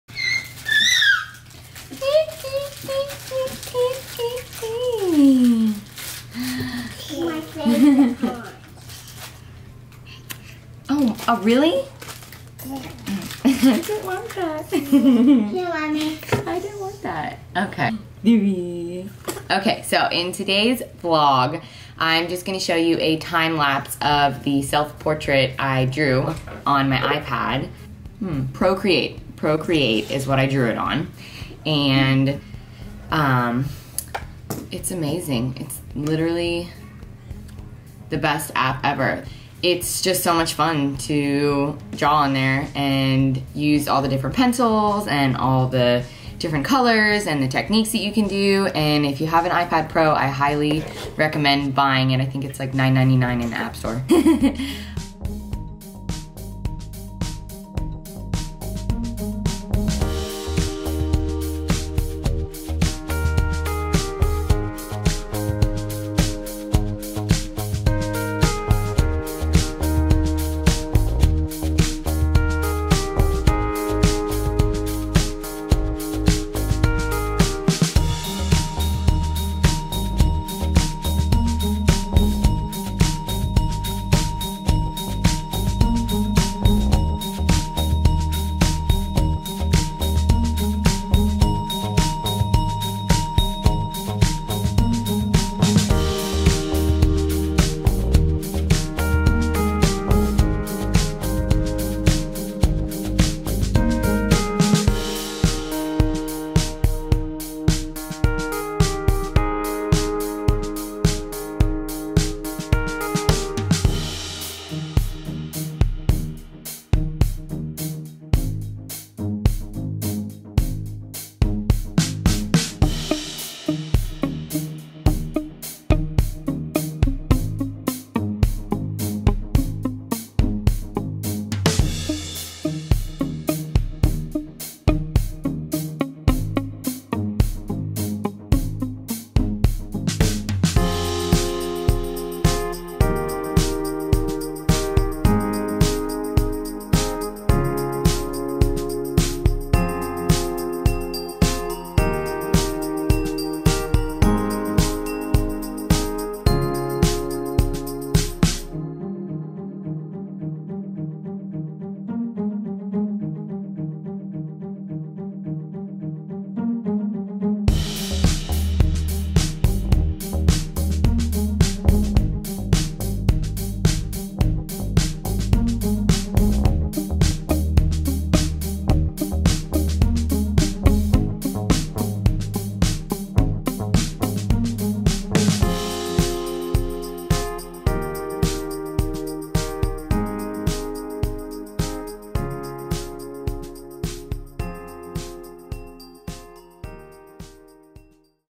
DIY - How to draw with procreate
In todays vlog, I show you how I draw a self portrait using procreate :-).